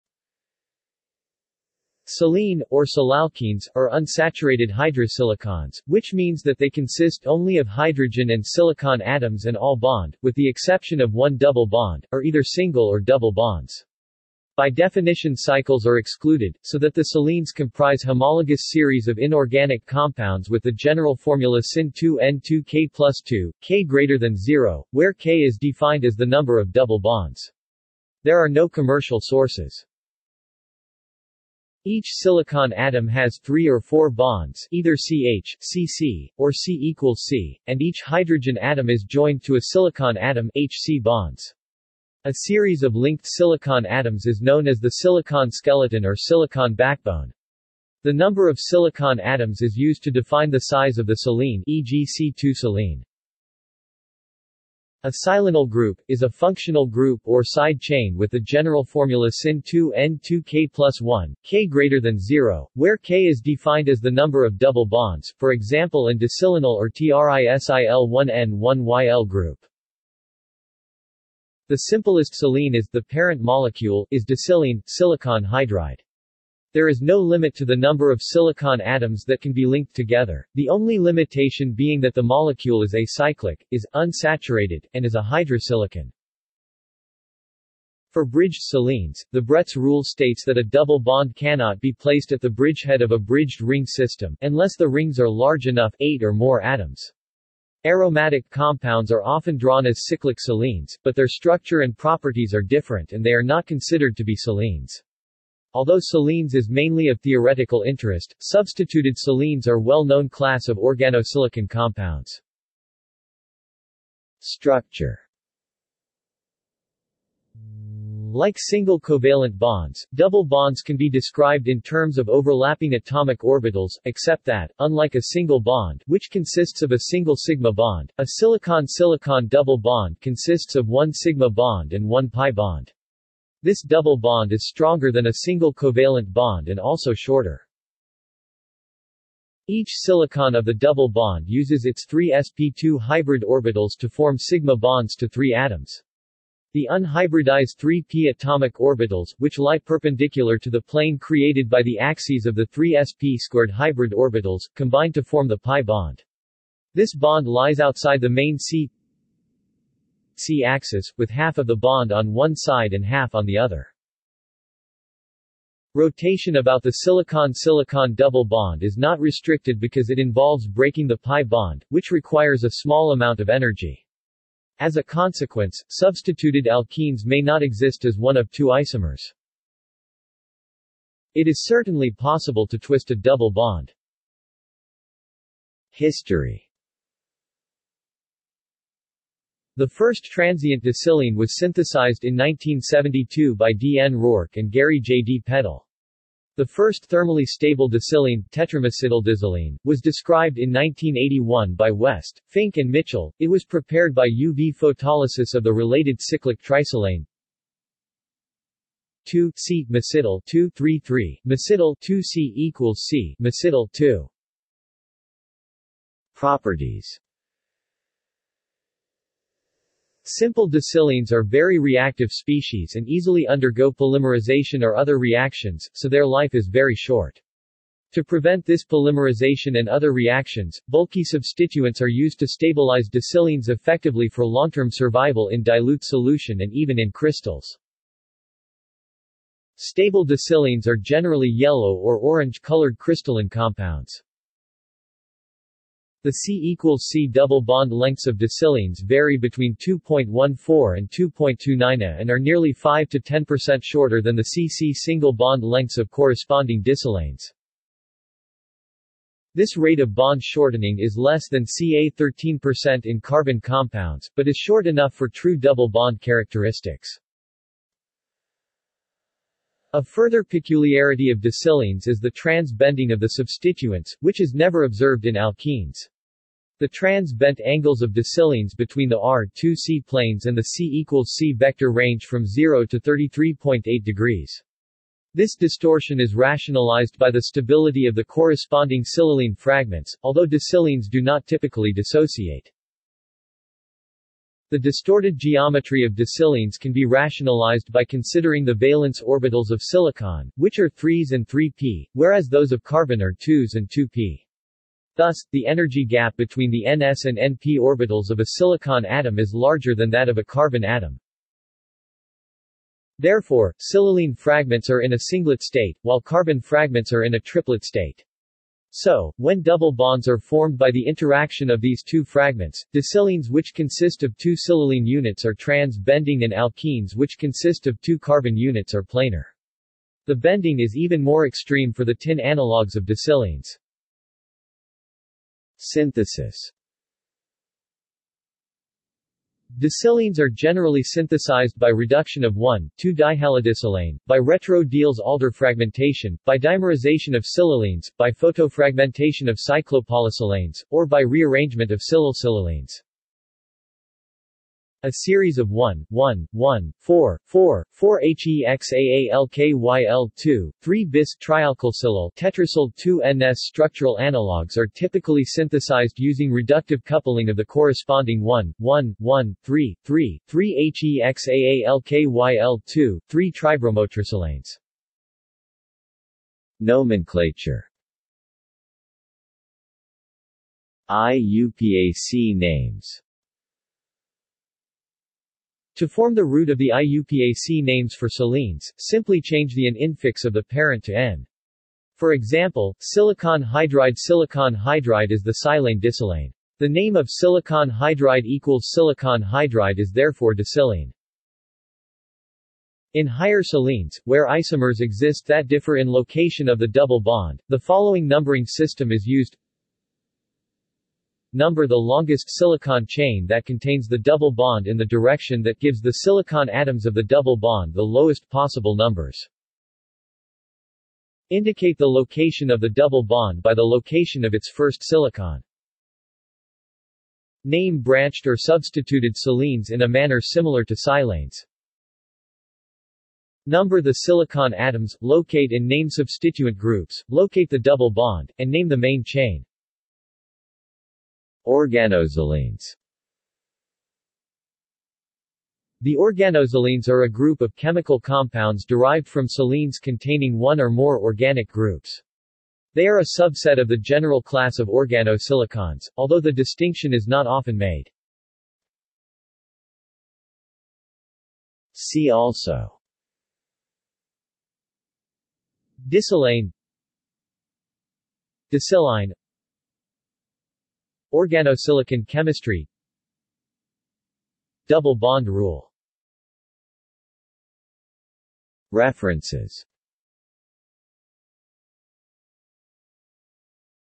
Silenes | Wikipedia audio article
This is an audio version of the Wikipedia Article:
Silenes

SUMMARY
Silene, or silalkenes, are unsaturated hydrosilicons, which means that they consist only of hydrogen and silicon atoms and all bond, with the exception of one double bond, are either single or double bonds. By definition cycles are excluded, so that the silenes comprise homologous series of inorganic compounds with the general formula SinH2n - 2k + 2, k  0, where k is defined as the number of double bonds. There are no commercial sources.
Each silicon atom has three or four bonds (either Si-H, Si-Si, or Si=Si), and each hydrogen atom is joined to a silicon atom (H-Si bonds). A series of linked silicon atoms is known as the silicon skeleton or silicon backbone. The number of silicon atoms is used to define the size of the silene (e.g. Si2-silene).
A silenyl group, is a functional group or side-chain with the general formula SinH2n - 2k + 1, k  0, where k is defined as the number of double bonds, for example an disilenyl or trisil-1-en-1-yl group.
The simplest silene is (the parent molecule) is disilene, Si2H4. There is no limit to the number of silicon atoms that can be linked together, the only limitation being that the molecule is acyclic, is, unsaturated, and is a hydrosilicon.
For bridged silenes, the Bredt's rule states that a double bond cannot be placed at the bridgehead of a bridged ring system, unless the rings are large enough (8 or more atoms). Aromatic compounds are often drawn as cyclic silenes, but their structure and properties are different and they are not considered to be silenes. Although silenes is mainly of theoretical interest, substituted silenes are well known class of organosilicon compounds.